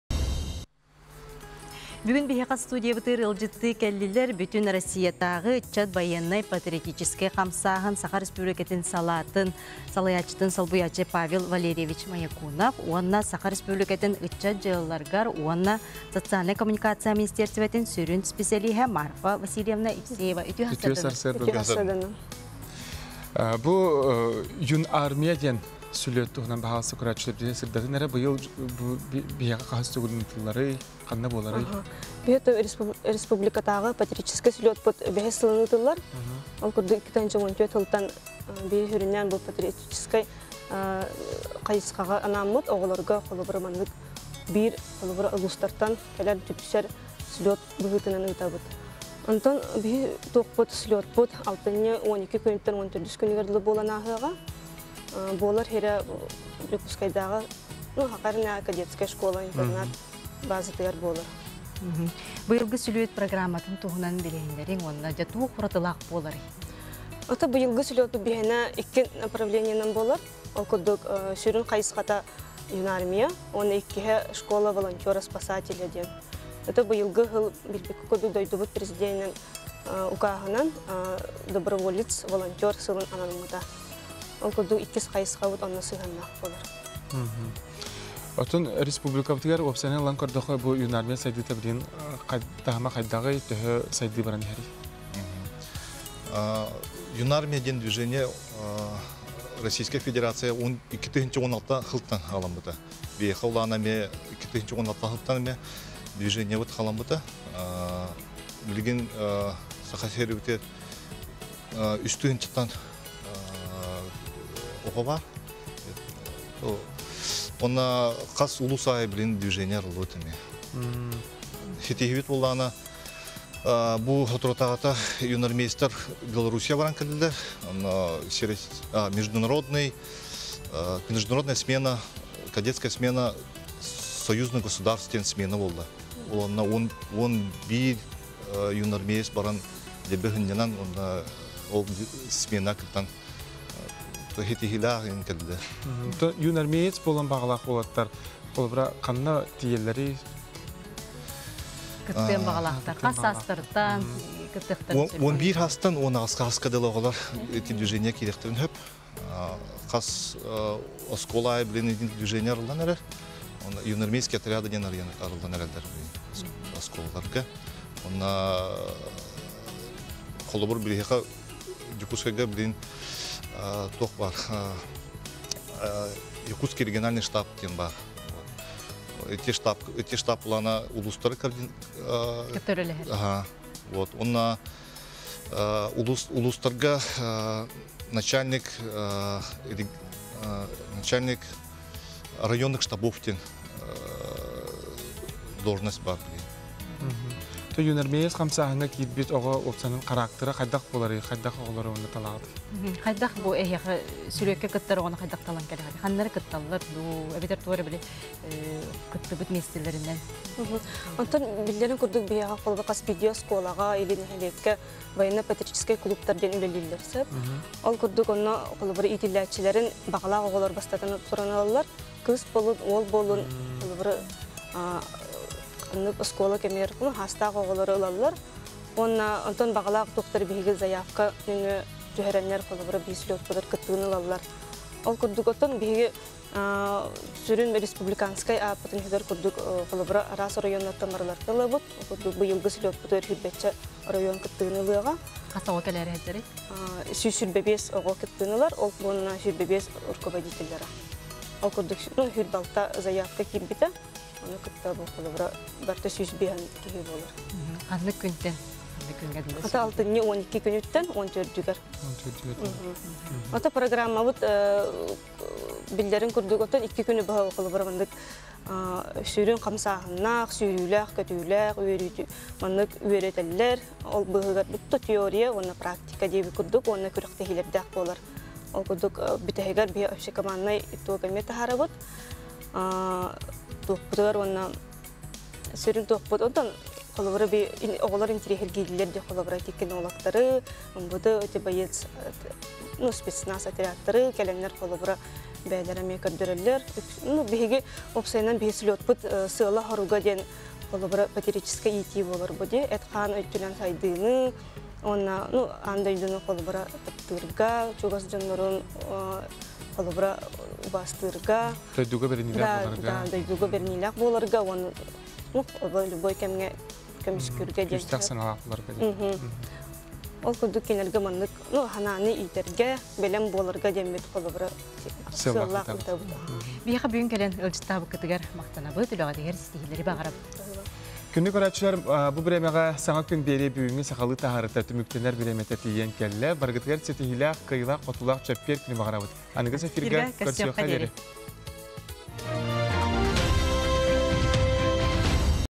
11 10 18 «ЮНАРМИЯ» БАЙЫАННАЙ ПАТРИОТИЧЕКСКАЙ ХАМСААҺЫН
Марфа ЕВСЕЕВА – СӨ ЫЧЧАТ ДЬЫАЛАЛАРЫГАР  УОННА СОЦИАЛЬНАЙ  КОММУНИКАЦИЯҔА МИНИСТИЭРИСТИБЭТИН СҮРҮННҮҮР ИСПЭССЭЛИИҺЭ
Павел МОЯКУНОВ - БҮТҮН РОССИЯТААҔЫ ЫЧЧАТ БАЙЫАННАЙ ПАТРИОТИЧЕСКАЙ ХАМСААҺЫН САХА СИРИНЭЭҔИ САЛААТЫН САЛАЙААЧЧЫТЫН СОЛБУЙААЧЧЫ